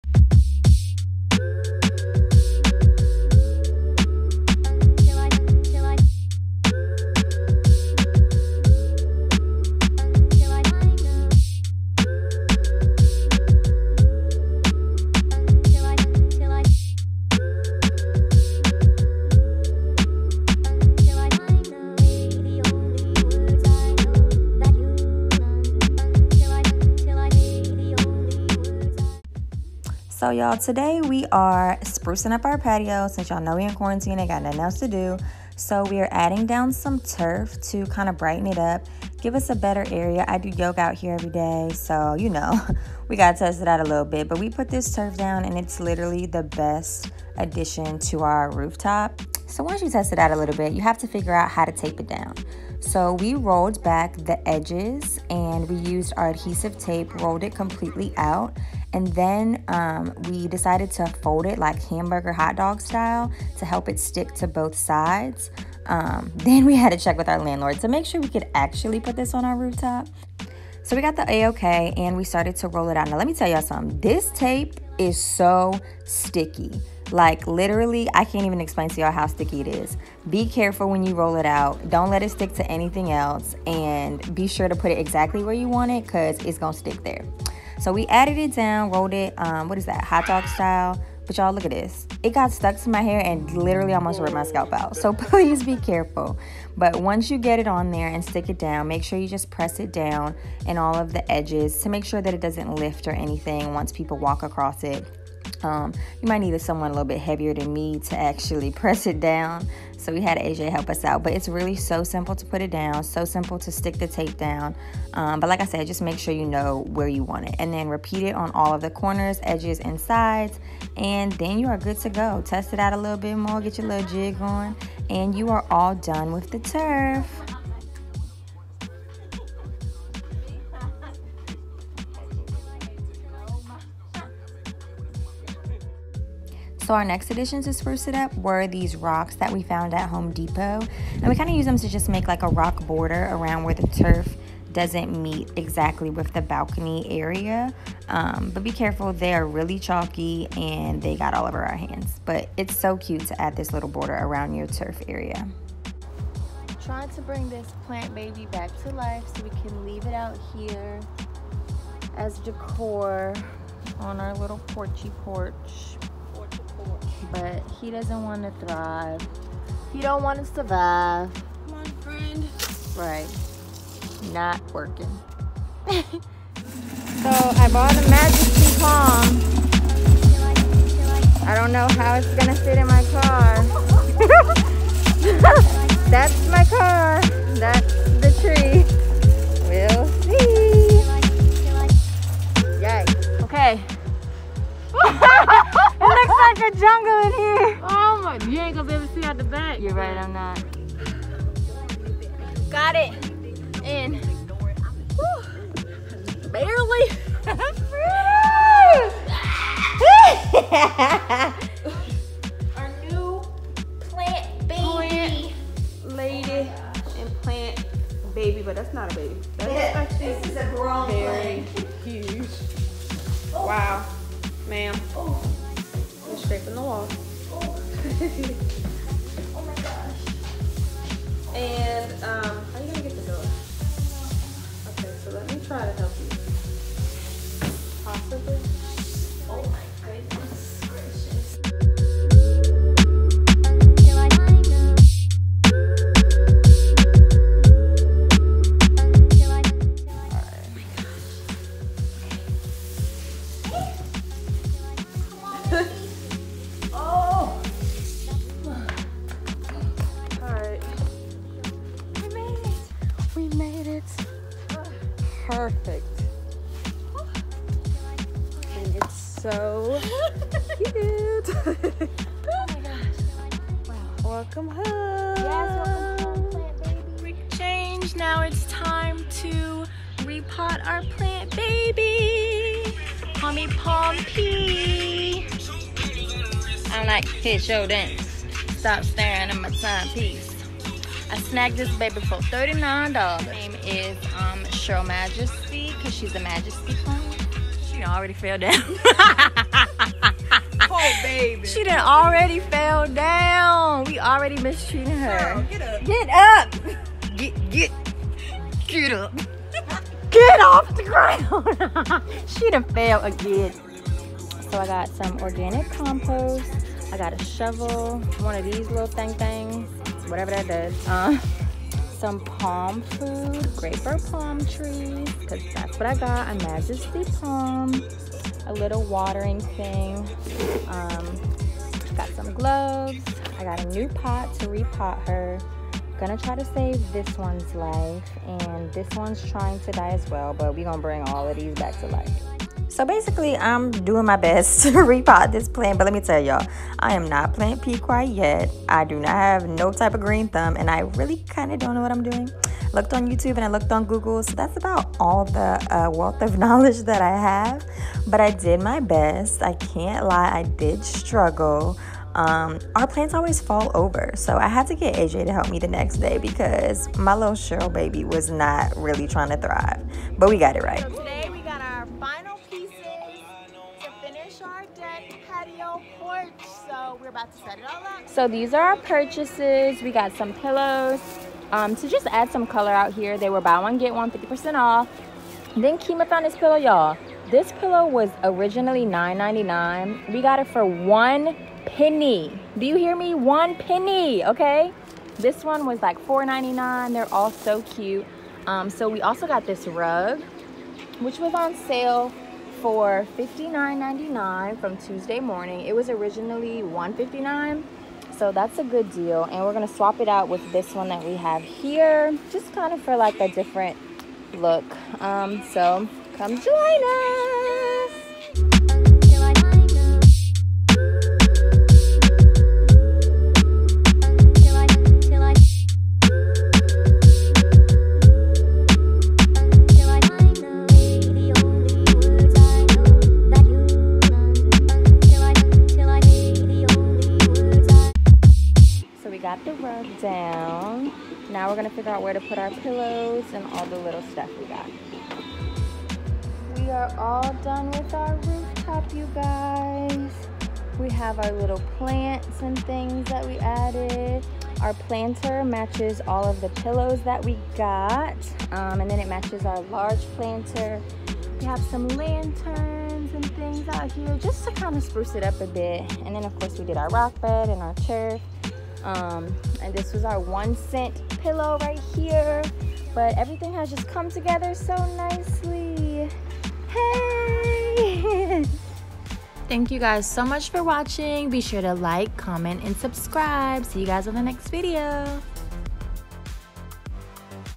Complete Rooftop Makeover!
As the weather has been heating up we’ve been dying to get outside! But Miss Rona has us stuck in the house, so we decided to spruce up our rooftop patio for a fun tropical vibe right at home.